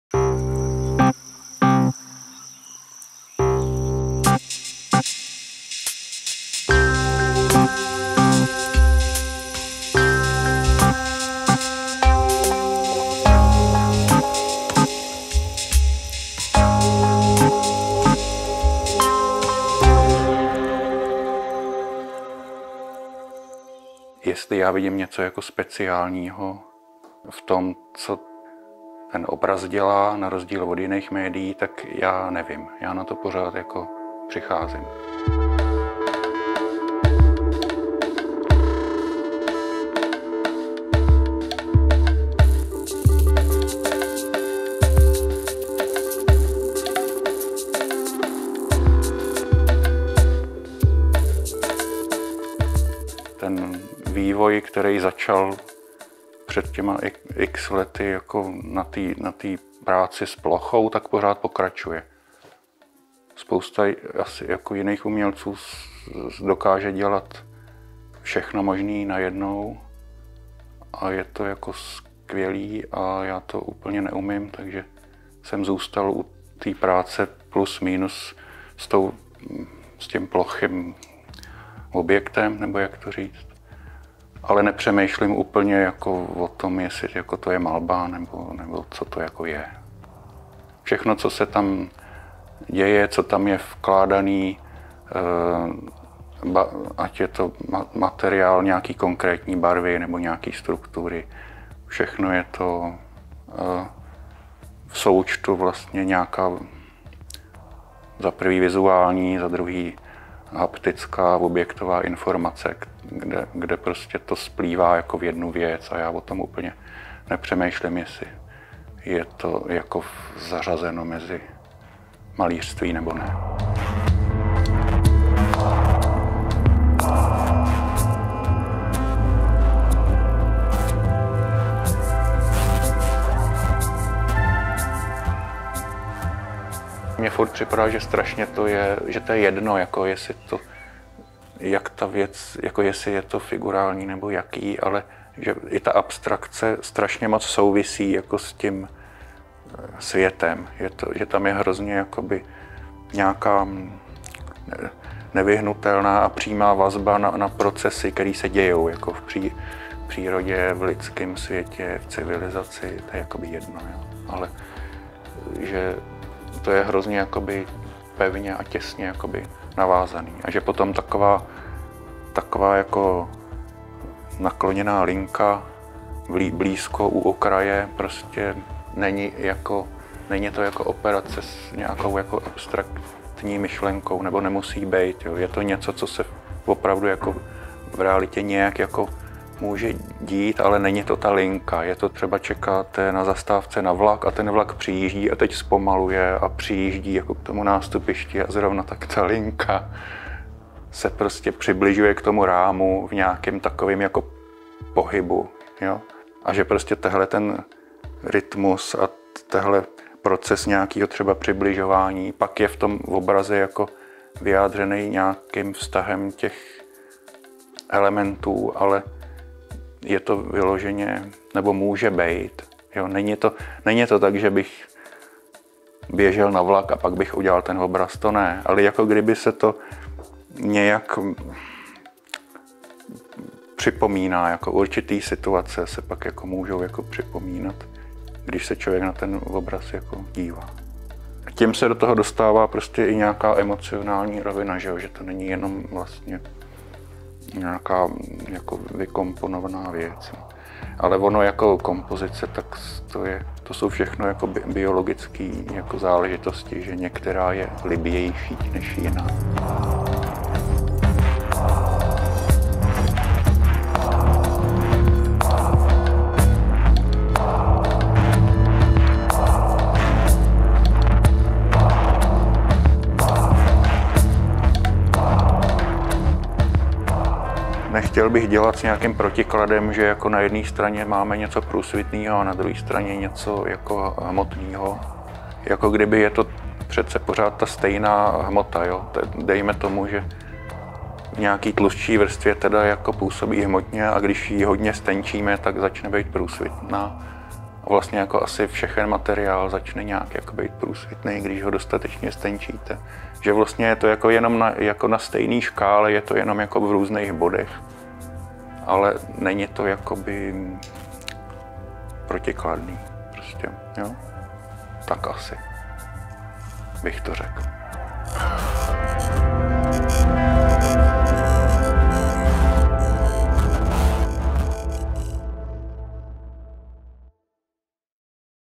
Jaromír Novotný: Ostatní se nemění
Výstava Jaromíra Novotného Ostatní se nemění představuje soubor nových prací autora v oblasti abstraktní monochromní tvorby. Díla – převážně obrazy, ale také fotogramy, sádry i dvě videa z posledního roku – jsou koncipována s ohledem na dispoziční řešení brněnského Domu umění. Můžeme je pozorovat samostatně, ale také v rámci jejich vzájemných vztahů, souvislostí a instalačního řešení jako jednoho kompozičního a významového celku.

Kurátorka: Marika Svobodová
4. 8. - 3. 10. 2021
Dům umění města Brna

Camera &amp; editing: Martin Dominik Kratochvíl
Graphic design: @anymadestudio